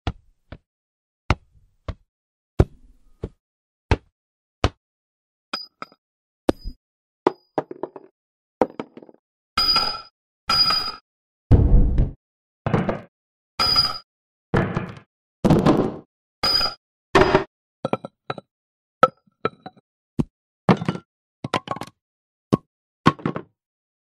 Impact Sound Effects - Foley Sound Effects Free Download
Foley impact sound effects for your projects. Get the free foley sound effects download below. Free foley sounds contain all 25 impact sound effects in this video.

Impact Sound Effects 2 is a collection of 197 foley impact sound effects. Includes objects impacting multiple surfaces such as wood, grass and concrete. In addition this foley sound effect pack also includes processed impact sound effects that give a more dramatic impact.